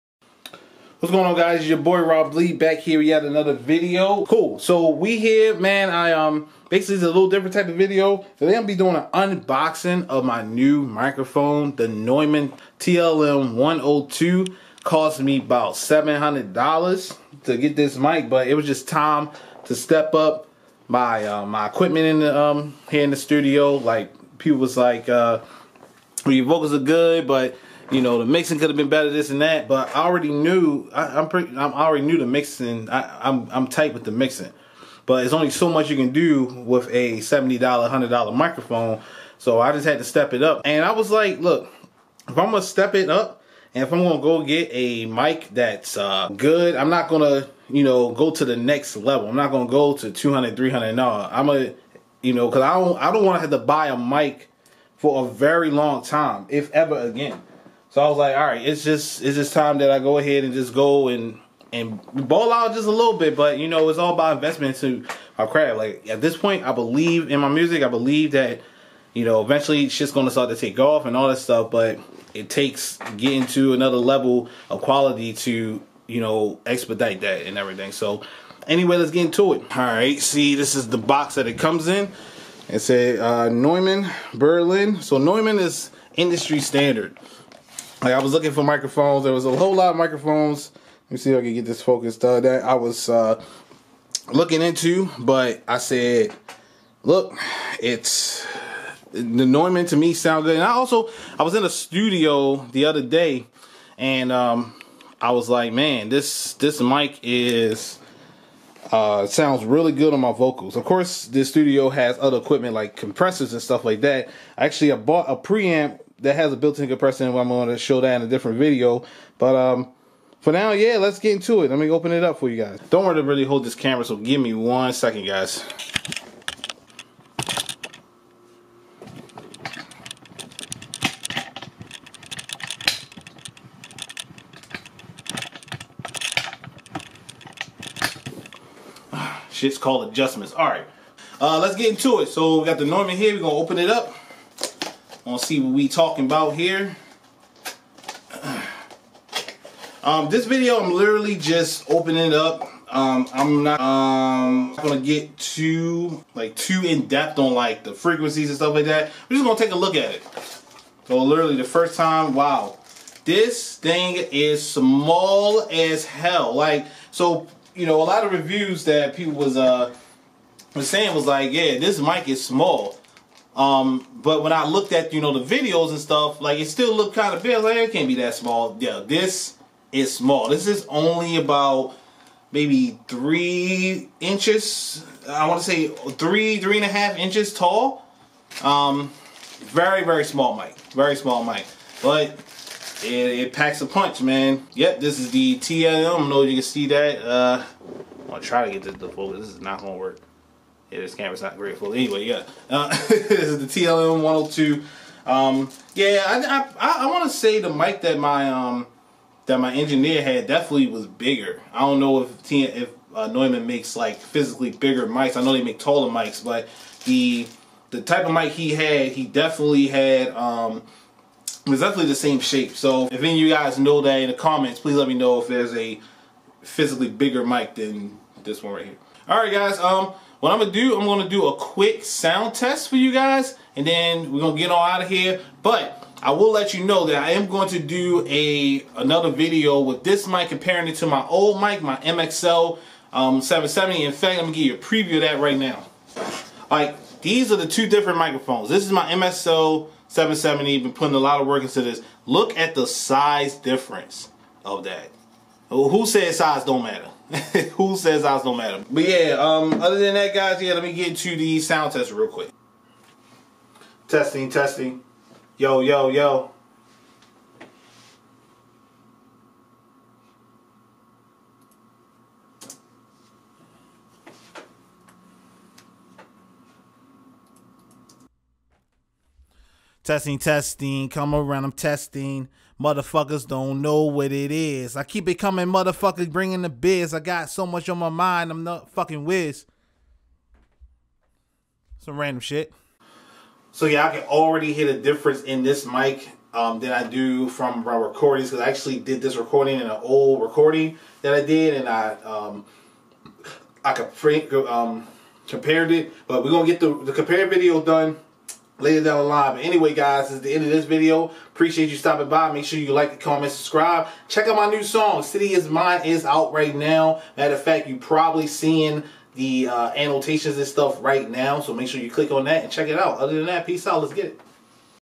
Neumann TLM 102 unboxing/ review
In this video I'll show you guys my new studio recording microphone. The Neumann TLM 102 is an industry standard microphone, used by many big time artist such as Drake. Its great for singing vocals, rap vocals and instruments.  I left some Amazon links down below if you're interested in purchasing